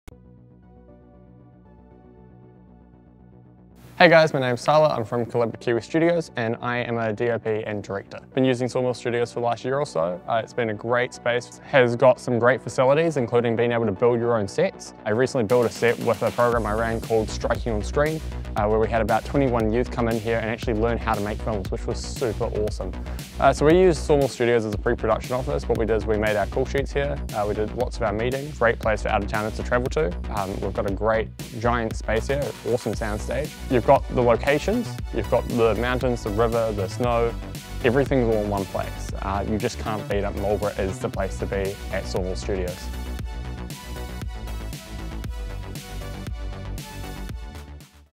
Sawmill Studios | Film Studio Marlborough, NZ - Tyler Redmond Filmmaker
Local D.O.P and Director Tyler Redmond shares his thoughts on Sawmill Studios and the many ways he has used the space so far including utilizing the available office space as a pre-production area to work on his most recent, and largest production yet, DIVE.

sawmillstudios.co.nz